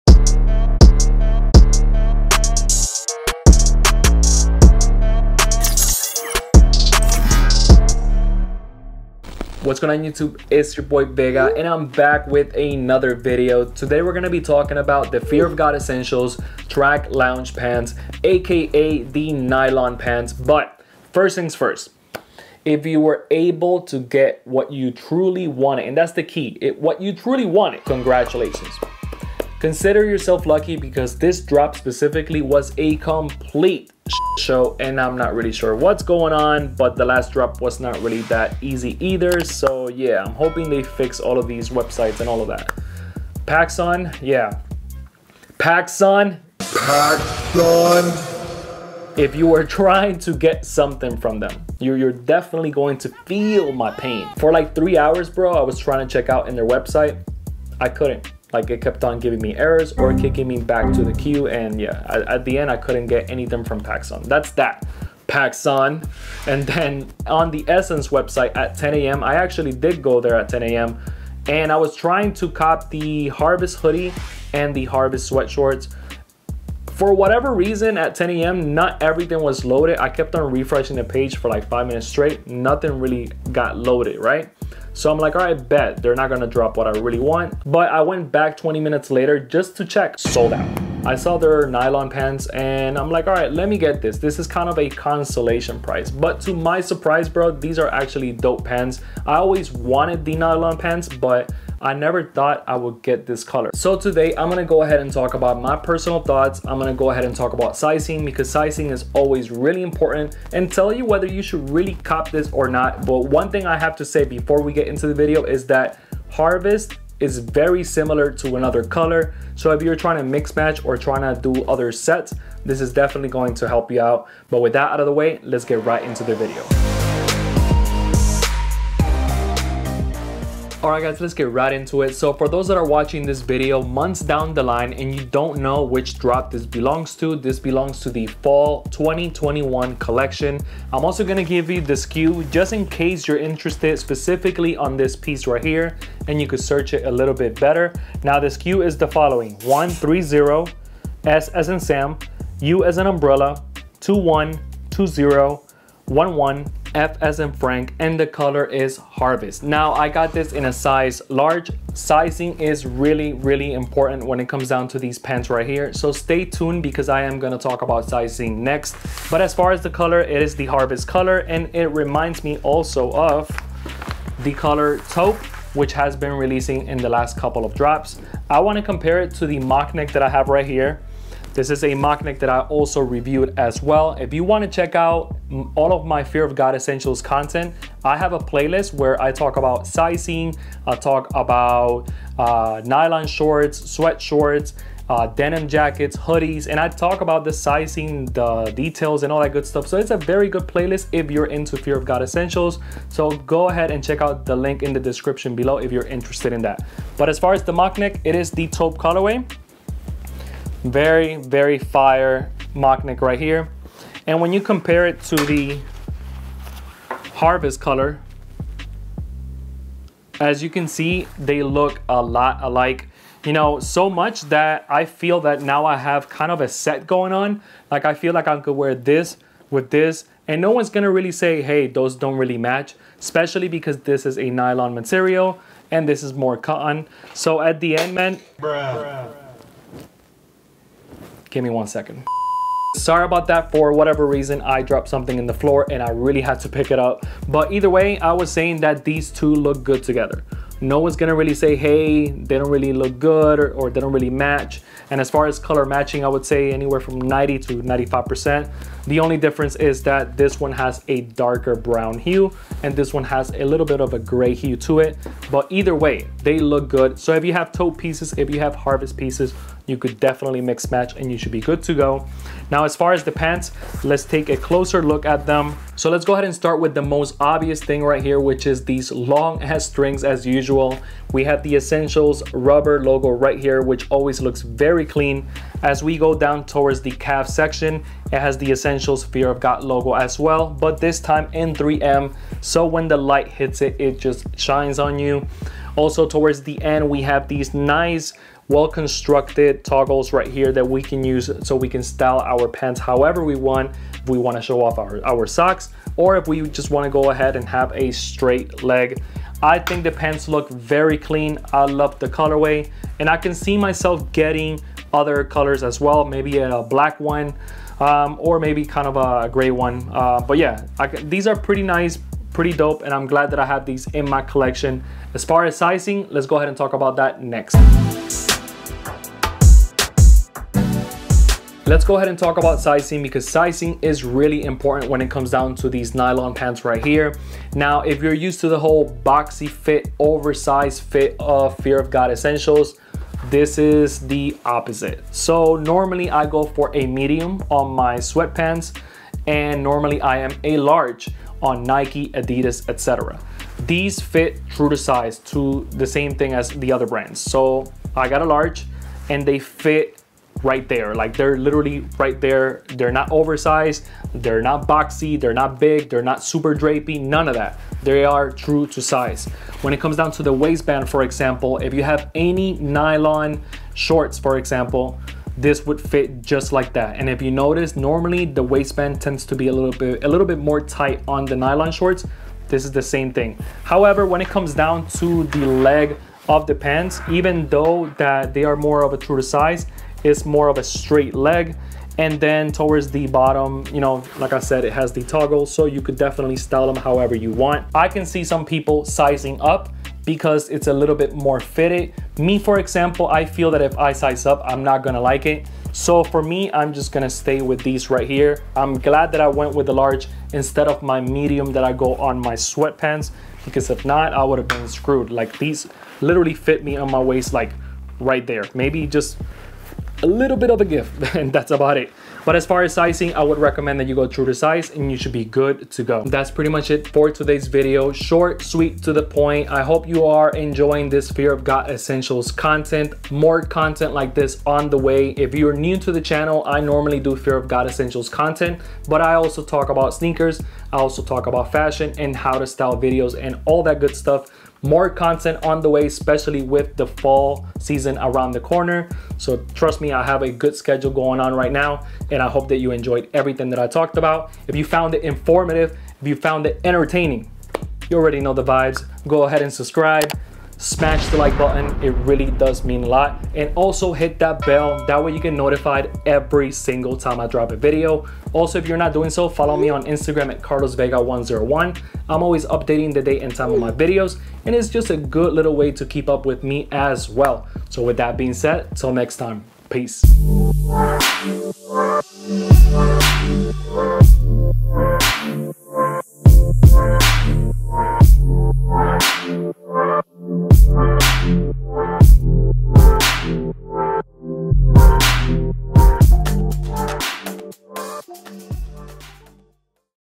Fear Of God Essentials Track Pants Review & Sizing (Harvest)
In this video I go over the Fear Of God Essentials Track Pants in the Harvest Color. This definitely a good piece to add to your Essentials Collection but sizing is really important with this one.